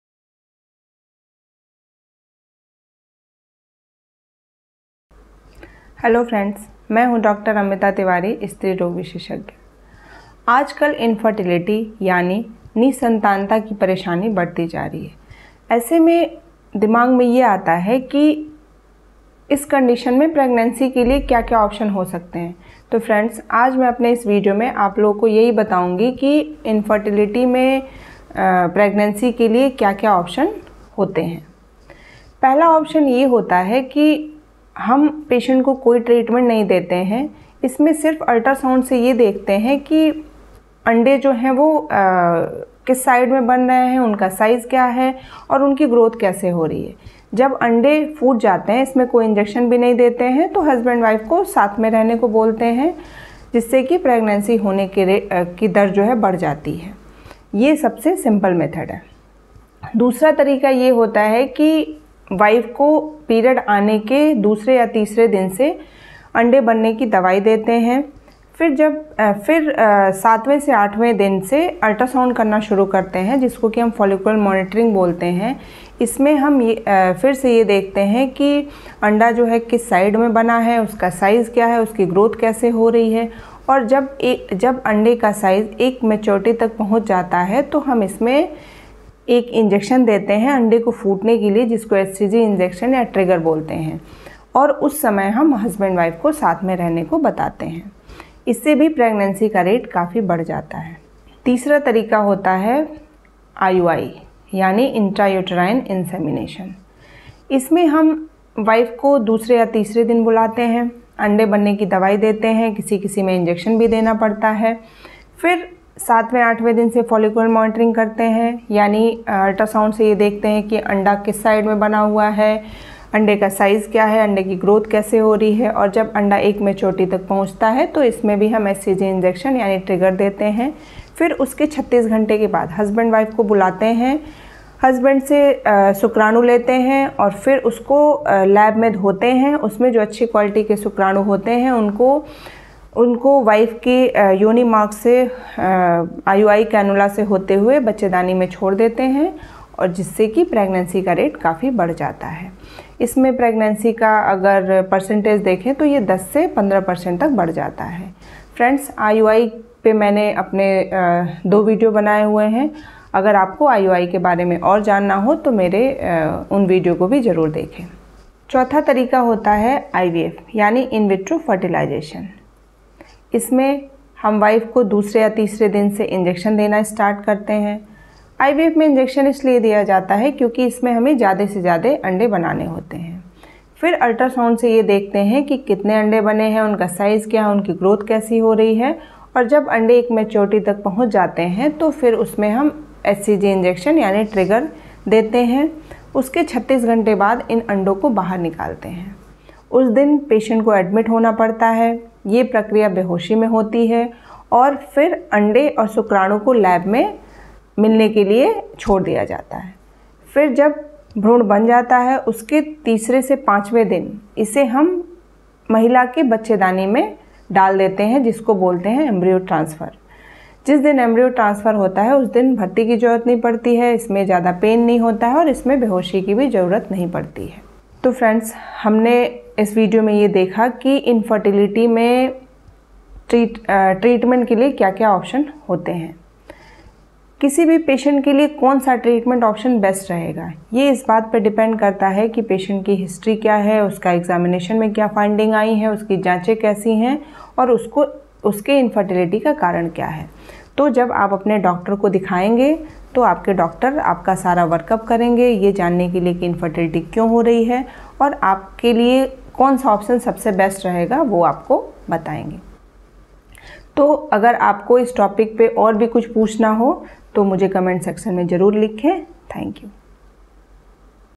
Infertility treatment options | Banjhpan mein pregnancy ke upay | Banjhpan ka ilaj in Hindi
Infertility treatment options | Banjhpan mein pregnancy ke upay | Banjhpan ka ilaj |
In this modern time many couples are suffering from infertility. The number of cases are increasing day by day. In this video I have discussed about infertility treatment for women.

Aaj kal dampatiyon mein baanjhpan ya nihsantanta ke cases badhte ja rahe hain. Is video mein maine banjhpan ke ilaj ke baare mein charcha ki hai.